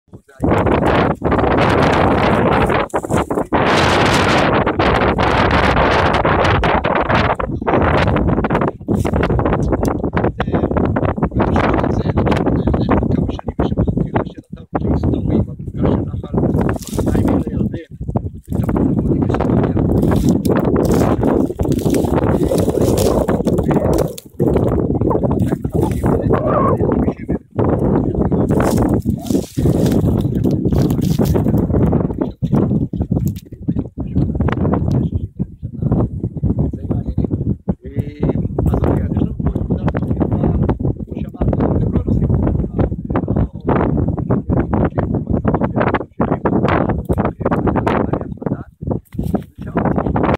Panorama from Hirbet Yarda, near Mishmar Hayarden, Israel
Panorama from Hirbet Yarda, near Mishmar Hayarden, Israel, site of bloody battle during Israeli War of Independence